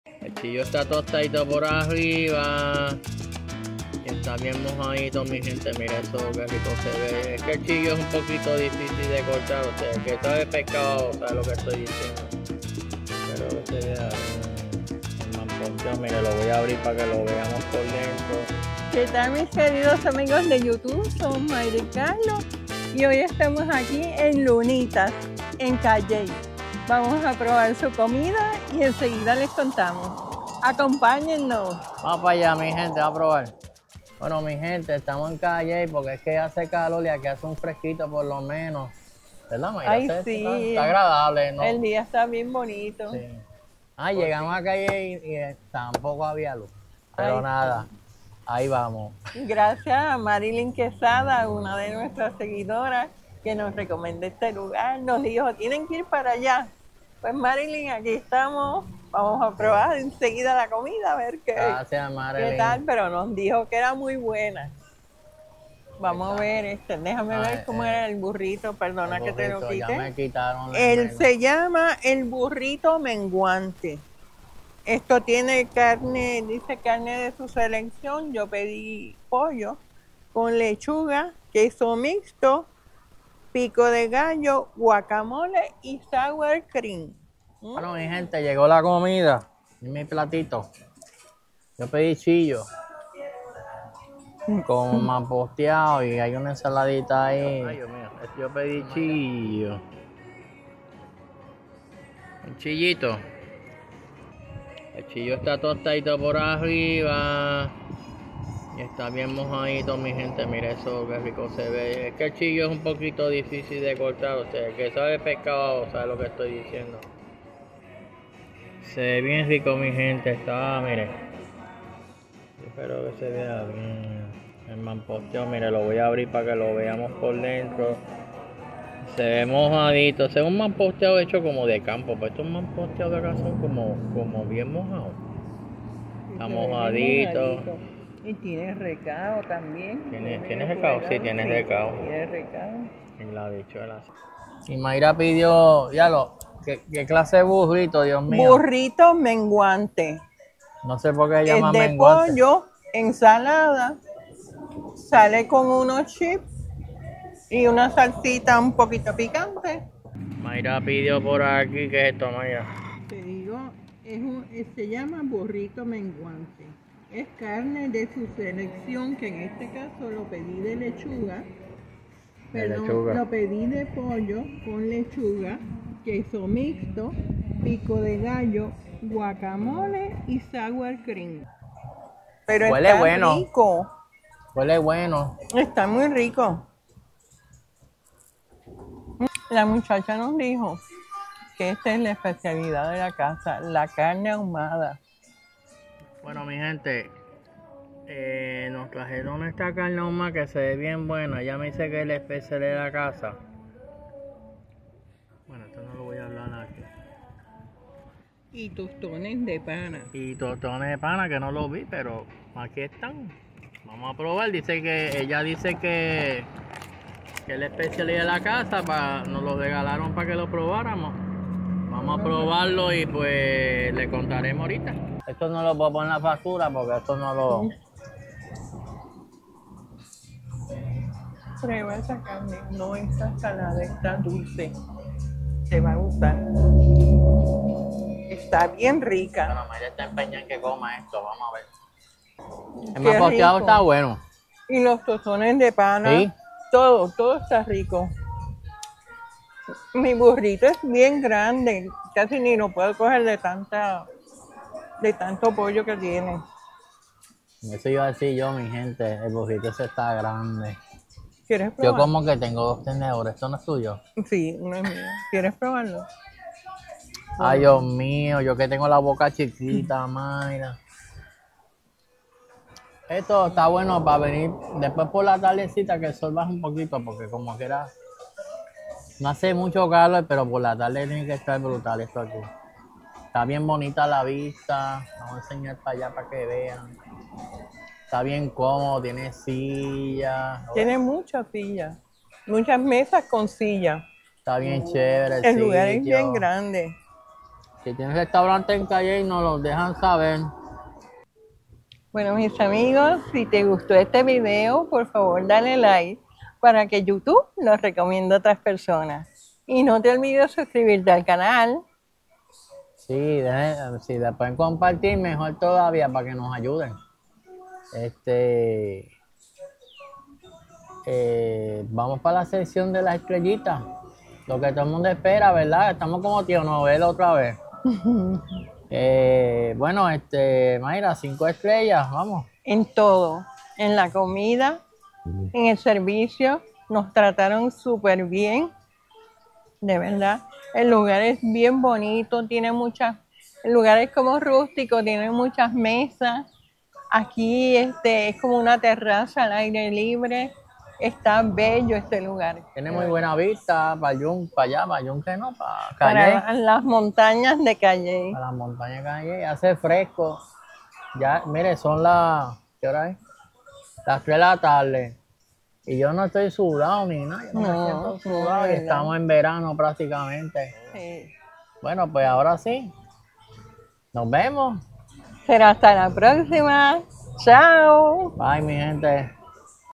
Cayey hermoso pueblo en las montanas de Puerto Rico PR
Restaurant Lunita que comida tan rica en una montana de Cayey nos dejo con ganas volver a visitar @BoricuaFoodiePR @boricuafull  @voyturisteando